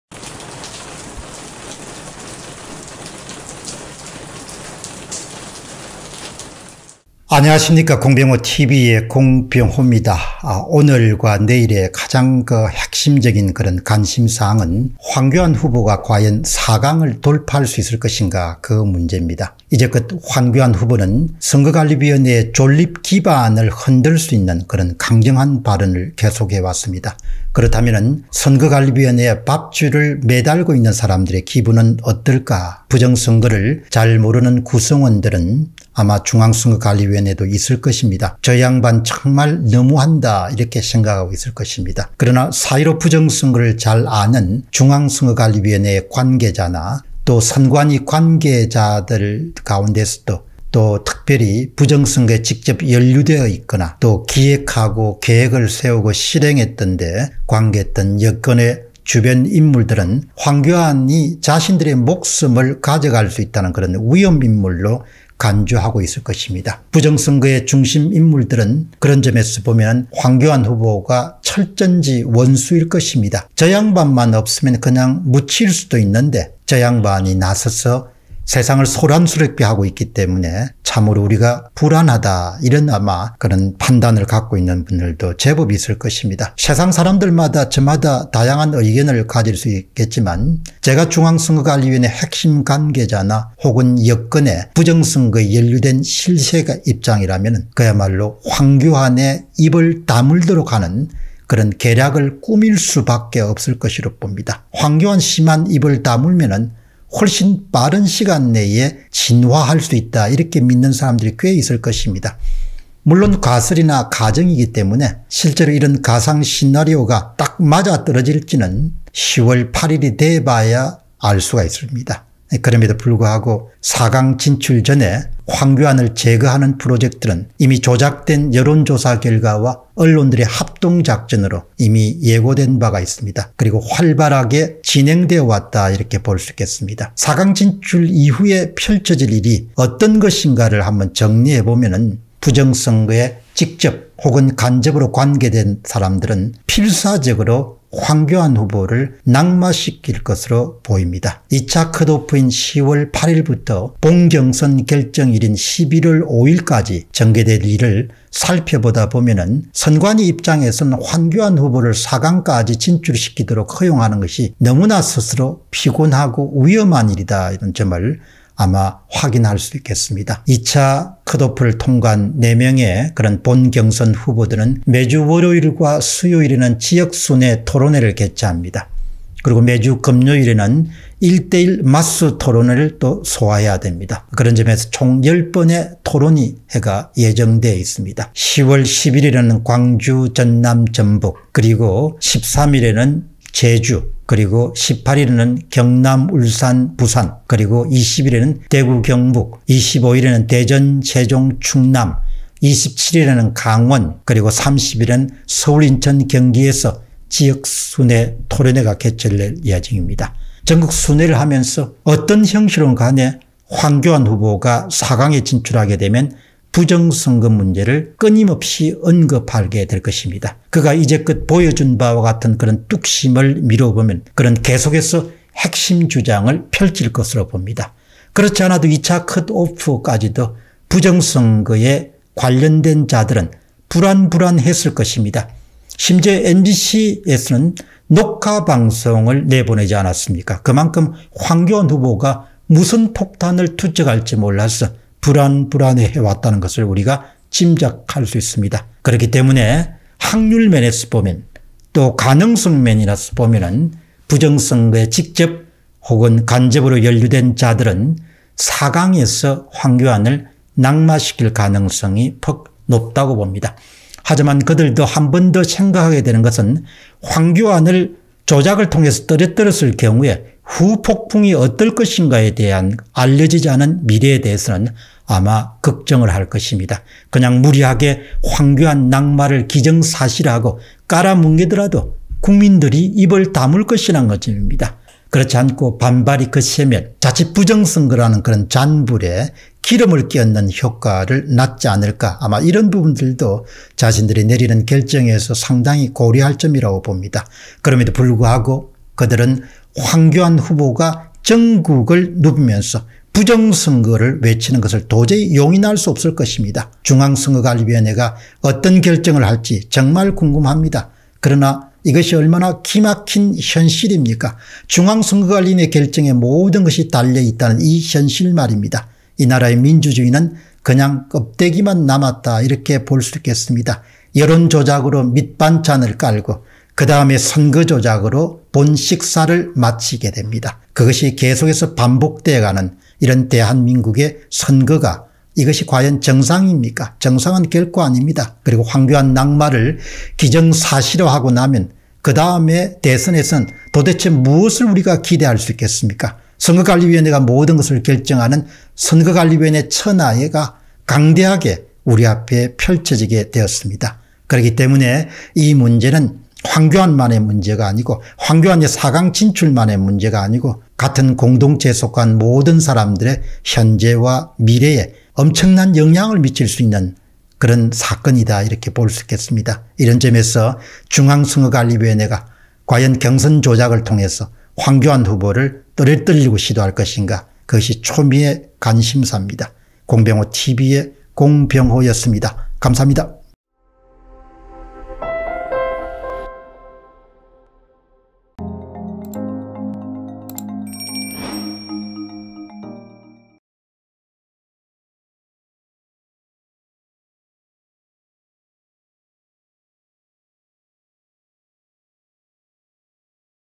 당신이 거대한 불법에 관계된 자라면 / 황교안, 살려두겠나 / 상대방 입장에서 생각해 보면, 용서받기 힘들 인물로 간주할 것 / 그러나 그렇게 했을 때 후폭풍도 [공병호TV]
Bitcoin : 3MTW849AkP2ftLeSK2teZHMmzLu7Z83s9U